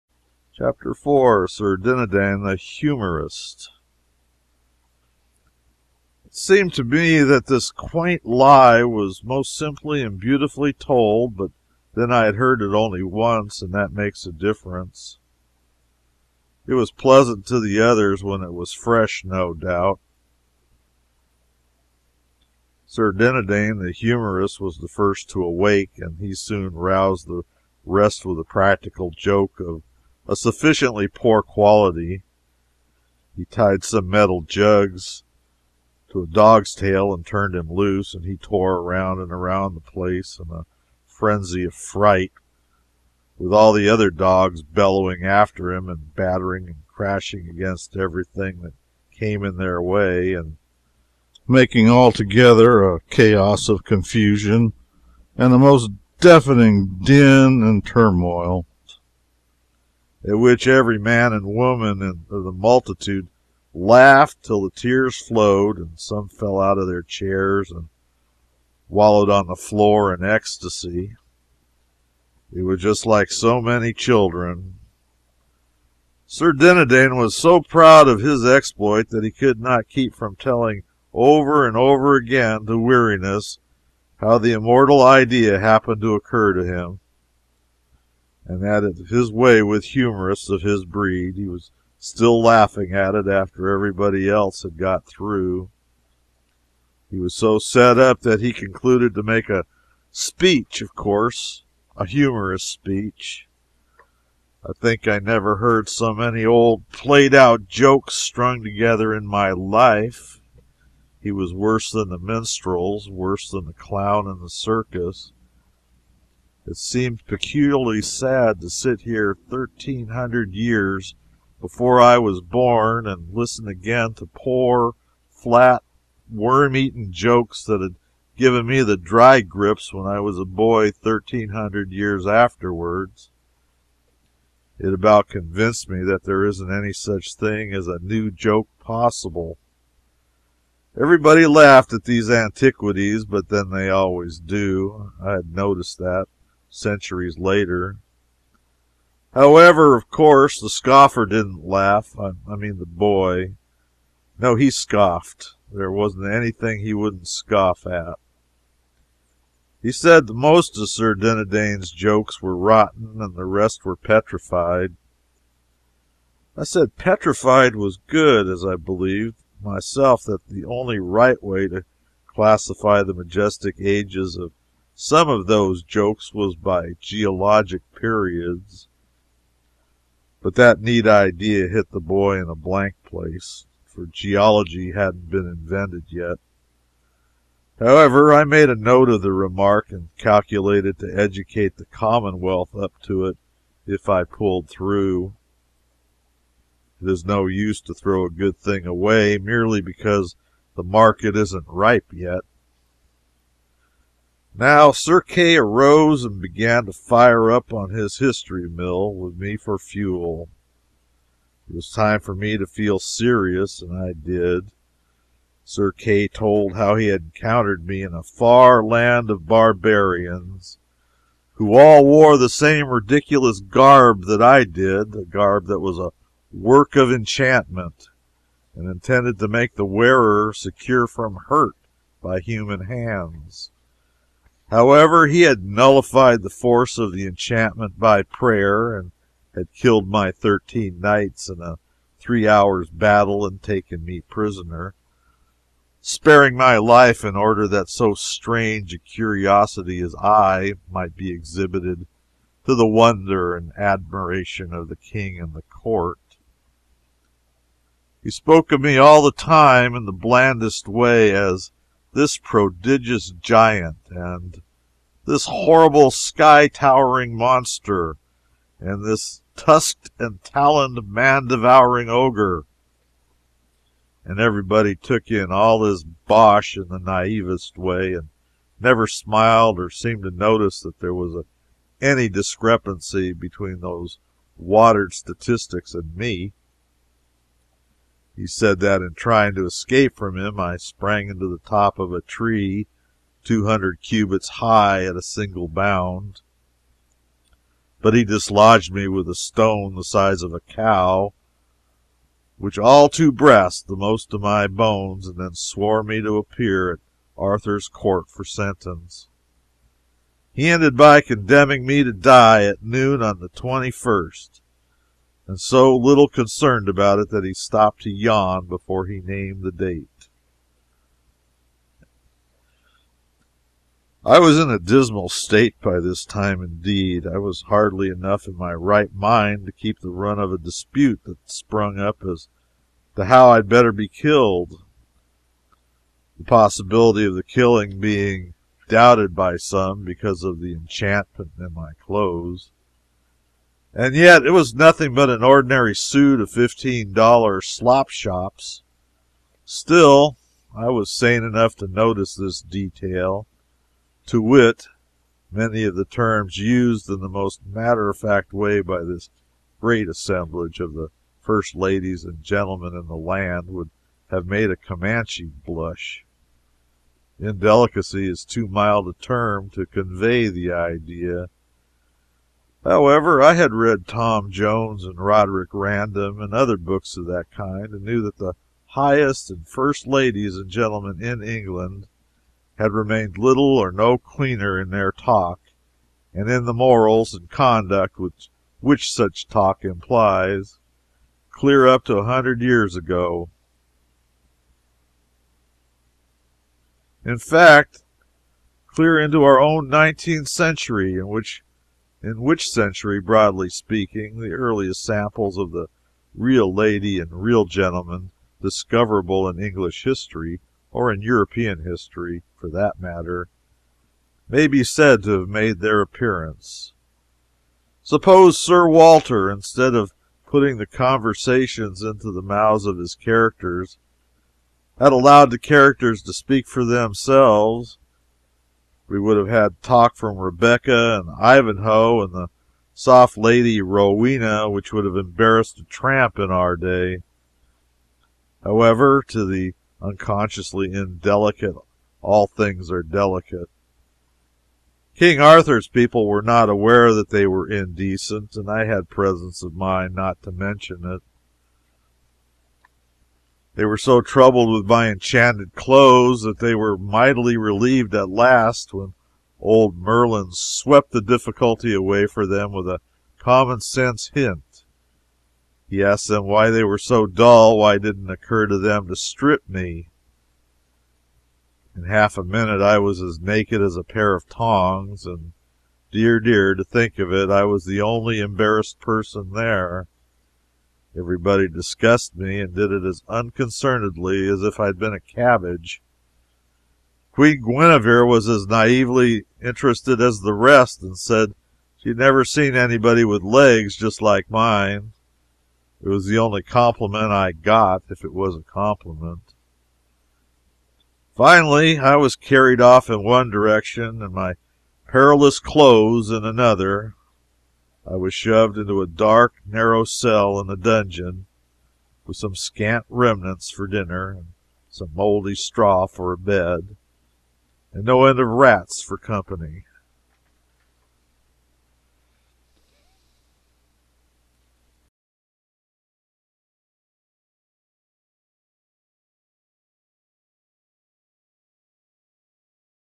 A Connecticut Yankee in King Arthur's Court: Chapter 4 - Sir Dinadan The Humorist
Hank is displayed before King Arthur and Queen Guinevere.  Sir Dinadan tells is tale of Hank's capture, describing him as a Sky Towering Monster with thirteen knights. Hank's clothing is described as enchanted.  He is stripped naked and thrown into a dungeon.